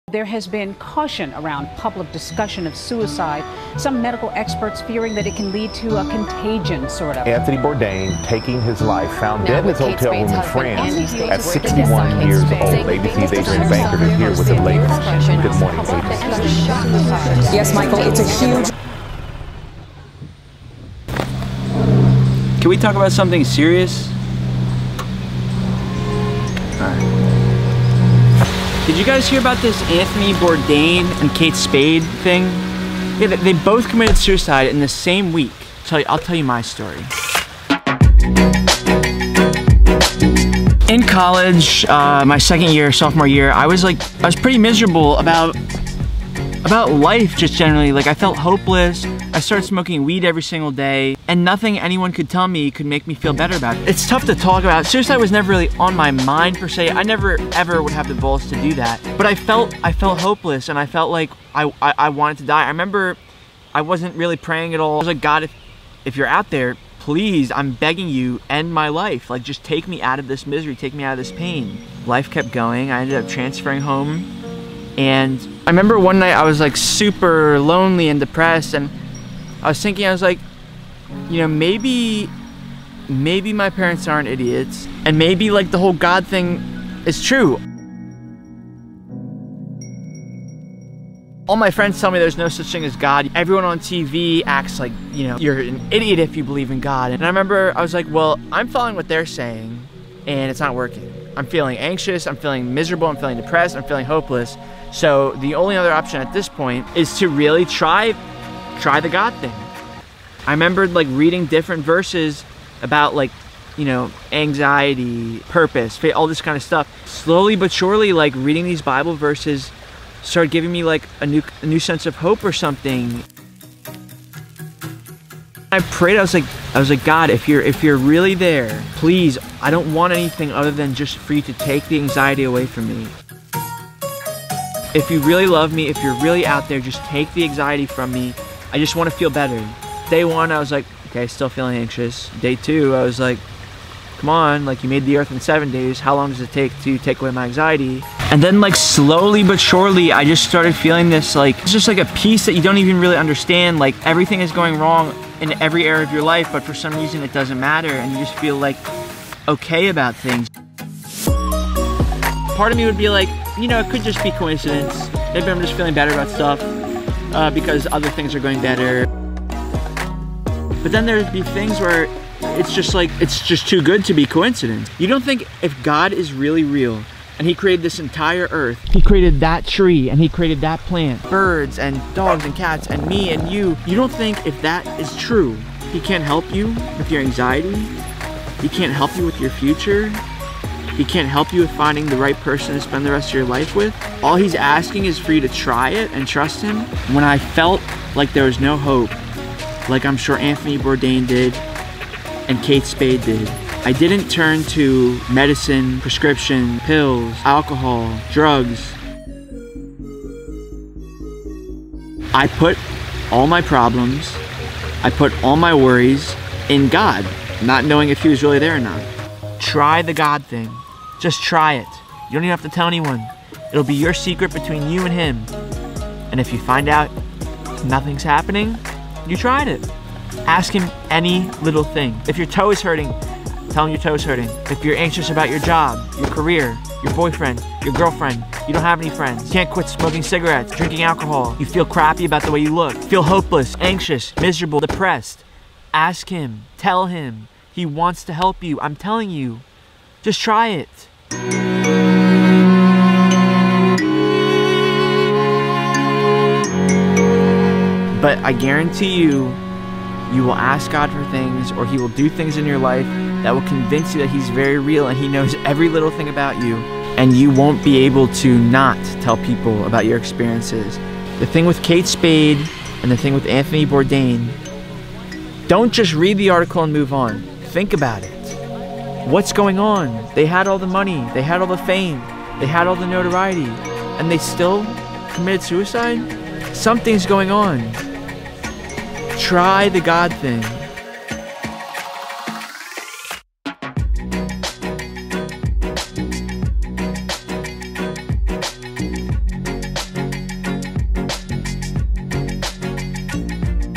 the day I asked God to end my life
the day I asked God to end my life...a very difficult and emotional video for me to make..but I felt like I had no choice. Kate Spade  and Anthony Bourdain both ended their lives in the same week, and suicide in the USA is more rampant than ever. Something is going on..and I tell you here what I did when I felt like I had no hope.